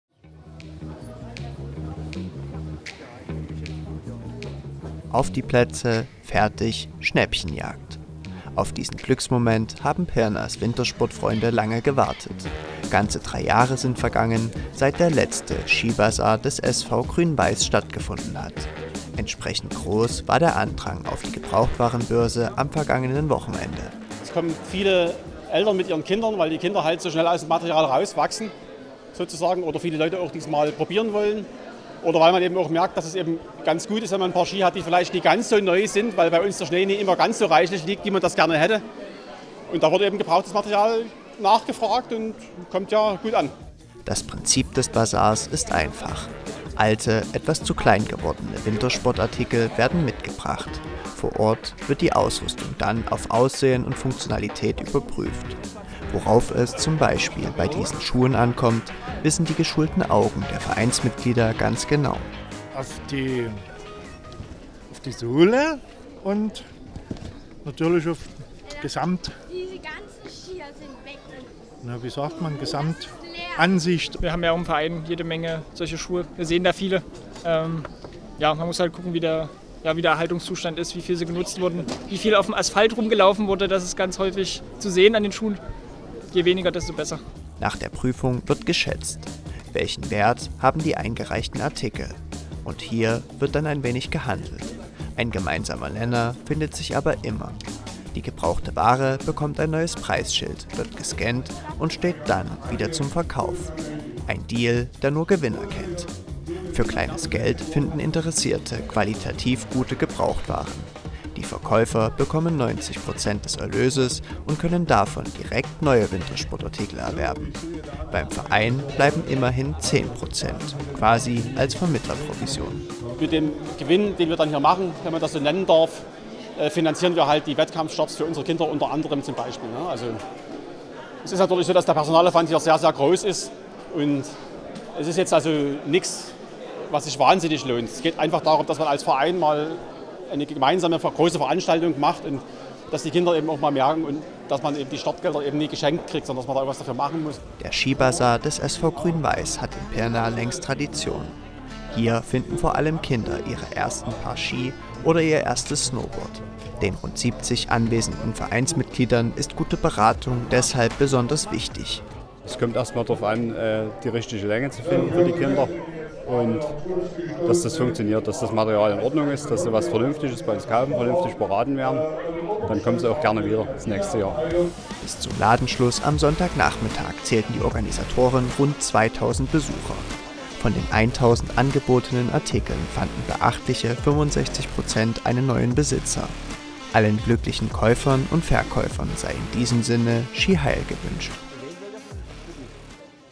Aus "Alt" mach "Neu" beim Skibasar des SV Grün-Weiss Pirna
Lange haben Pirnas Wintersportfreunde auf diesen Moment gewartet. Am vergangenen Wochenende war es nun endlich so weit: Der Skibasar des SV Grün-Weiss ist zurück. Die Tauschbörse erfreute sich großer Beliebtheit. Über  gebrauchte 1.000 Artikel wurden zum Wiederverkauf angeboten. Das ein oder andere Schnäppchen fand schnell einen neuen Besitzer.